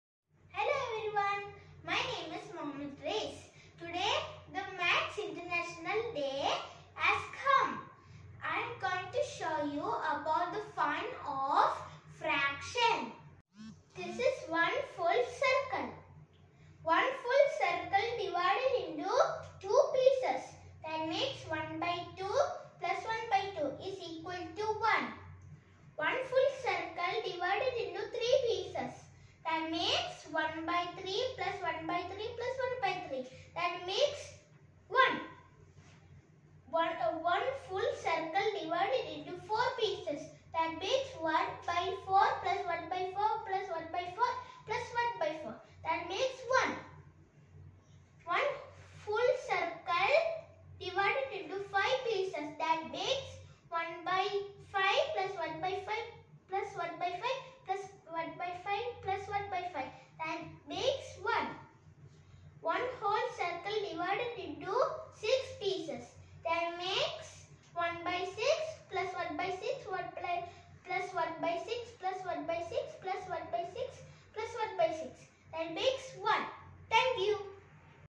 World Maths Day # Fun in Fraction # Maths project for kids
Fun in Fraction

1 circle divided into equal parts give 1.

1/2 + 1/2 =1
Here denominator is same hence it should be kept same and the numerator to be added (sum)

1/2 +1/2 =  2/2 = 1